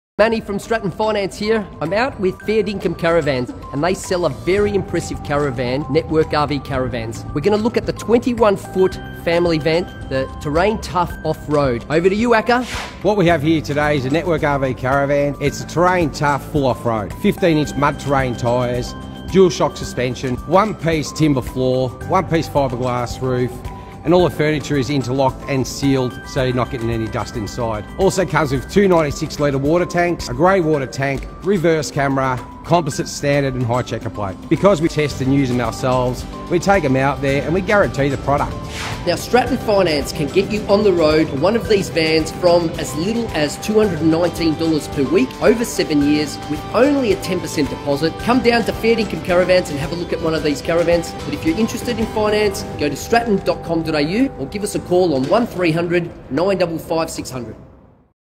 Stratton Finance - Fair Dinkum Caravans
The Terrain Tough by Network RV Caravans is a 21 foot, full off-road family van. Stratton Finance have partnered with Fair Dinkum Caravans NSW & VIC to give you the best deals on your next caravan and get you on the road with one of these rigs for as little as *$219 per week!

Finance this van with Stratton Finance at a Fair Dinkum Caravans dealership today!